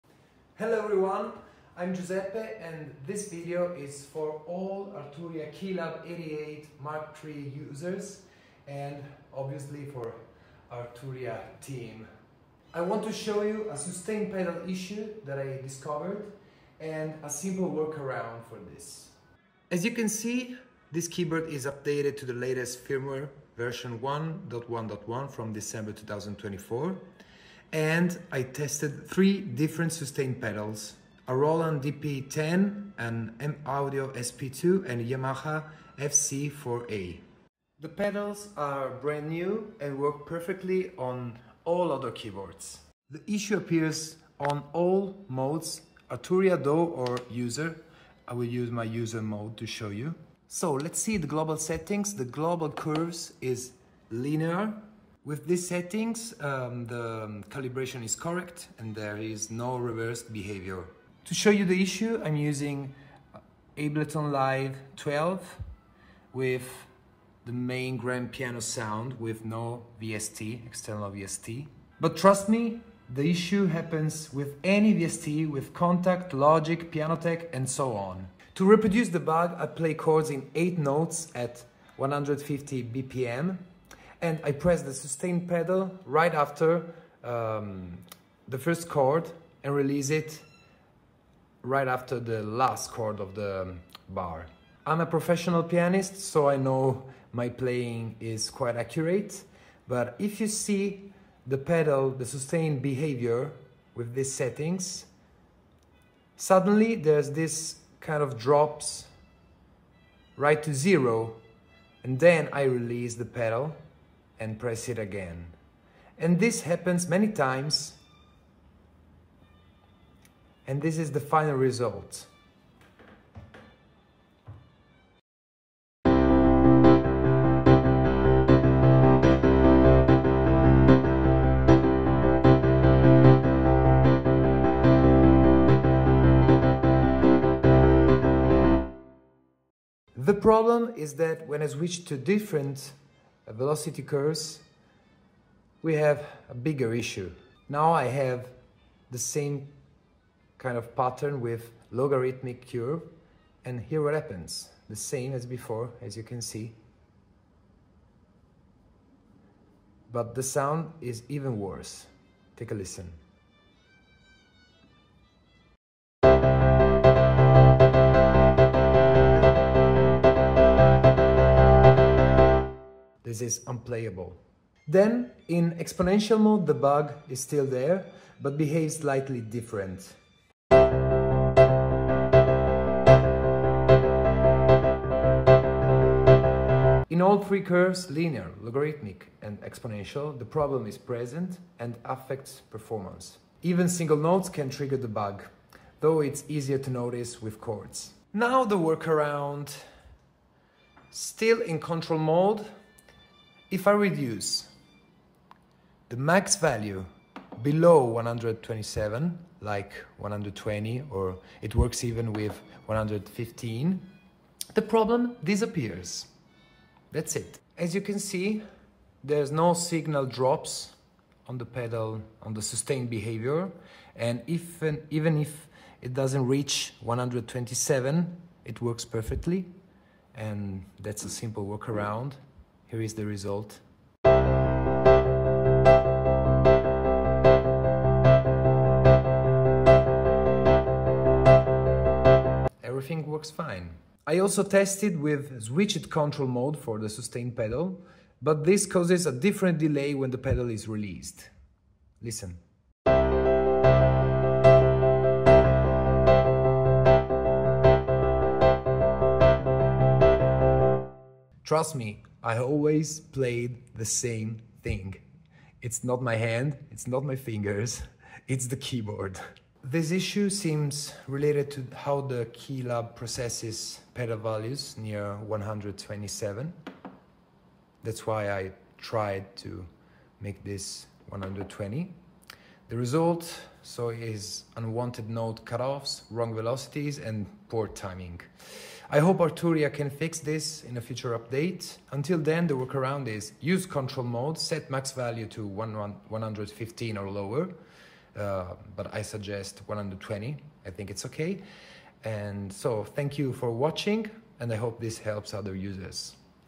KeyLab 88 MkIII Sustain Pedal Bug (Firmware 1.1.1) – How to Fix It (MIDI + Audio Demo)
⚠️ Sustain pedal issue on Arturia KeyLab 88 MkIII – Firmware 1.1.1
🎹 In this video, I show a critical bug I found when using the sustain pedal on the KeyLab 88 MkIII in User Mode, and I explain a workaround that helped me fix it.

🔍 Symptoms:
 • MIDI sustain signal drops to 0 while the pedal is still pressed
 • This causes sustain cutoff, delayed notes, and random velocity spikes
 • Problem happens on Linear, Logarithmic, and Exponential key velocity curves
 • More frequent on chords played at fast tempos (e.g. 150 BPM)
 • Present in Ableton Live 12, but also tested and confirmed in Kontakt, Logic, and other VSTs

🔧 Setup:
 • KeyLab 88 MkIII – Firmware 1.1.1
 • Pedals tested: Roland DP-10, M-Audio SP-2, Yamaha FC-4A (all new and working on other keyboards)
 • Pedal Mode: Control (calibrated)
 • Key Velocity Curve: Linear / Logarithmic / Exponential

✅ Workaround (Temporary Fix):
 1. Keep pedal in Control Mode (for half-pedal support)
 2. In Sustain Pedal Settings, lower Max Value from 127 to something like 120 or even 115
➡️ This stabilizes the signal and stops random drops to zero

Files:
 • MIDI curve recordings showing the dropouts
 • Audio examples of the timing/velocity issues
 • The working fix with adjusted Max Value